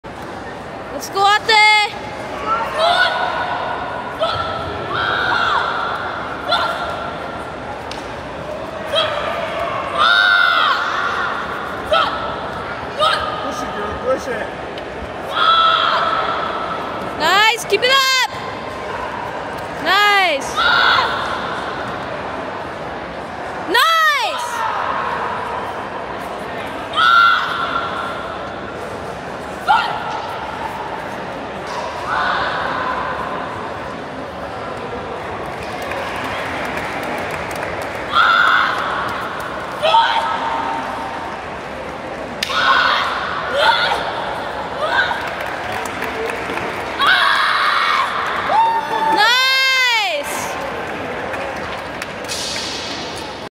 Lynlyn I. Open Weapons Day 1 [ WKC Canadian Nationals 2024 ]
The WKC Canadian National Team Tryouts from May 17-19, 2024.